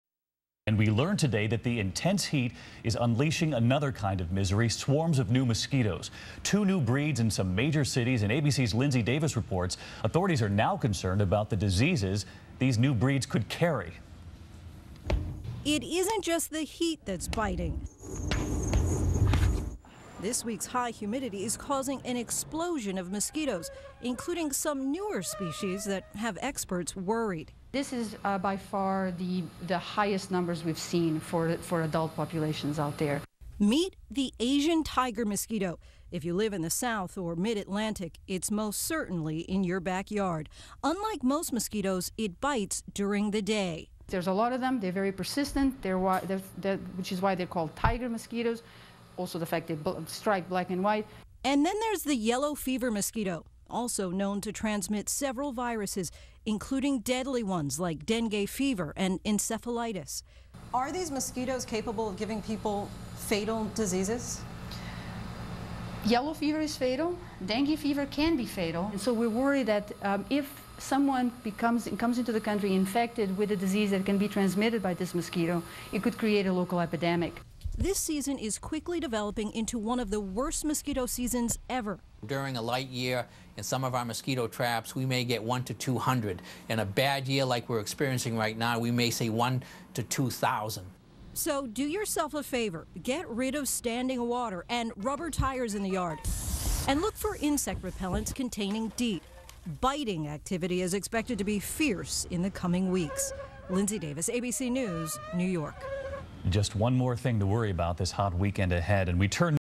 New Mosquito Breeds and what they could carry
Asian Tiger Mosquito -  She will bite you in broad daylight as well as at night - call her the SUPER mosquito... and she is HERE, and sha is HERE NOW.

As well as the Yellow Fever Mosquito... transmits Dengue Fever and Encephelitis...
Yellow Fever is fatal.
Dengue fever CAN be fatal.